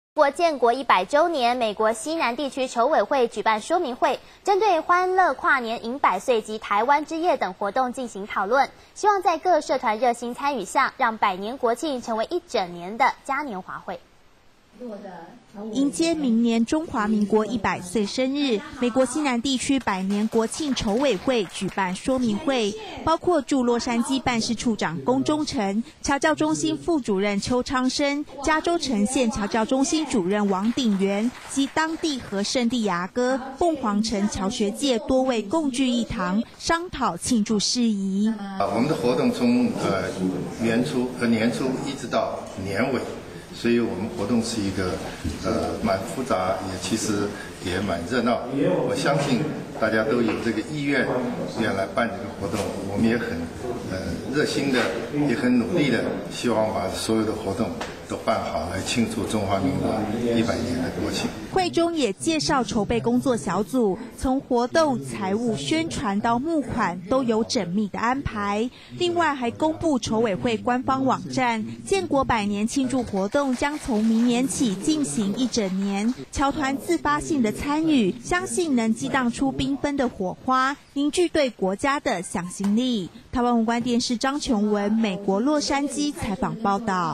建國百年美西南地區慶祝活動 - 僑社新聞 08-07-10
美國 洛杉磯
建國百年美西南地區慶祝活動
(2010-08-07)
迎接明年中華民國一百歲生日
美國西南地區百年國慶籌委會
舉辦說明會
包括駐洛杉磯辦事處長龔中誠
僑教中心副主任丘昌生
加州橙縣僑教中心主任王鼎元
及當地和聖地牙哥 鳳凰城僑學界
多位共聚一堂 商討慶祝事宜
我們的活動從年初一直到年尾
所以我們活動是一個蠻複雜
其實也蠻熱鬧
我相信大家都有這個意願
來辦這個活動
我們也很熱心地也很努力地
希望把所有的活動都辦好
來慶祝中華民國一百年的國慶
會中也介紹籌備工作小組
從活動 財務 宣傳到募款
都有縝密的安排
另外還公佈籌委會官方網站
建國百年慶祝活動
將從明年起進行一整年
僑團自發性的參與
相信能激盪出繽紛的火花
凝聚對國家的向心力
台灣宏觀電視 張瓊文
美國洛杉磯 採訪報導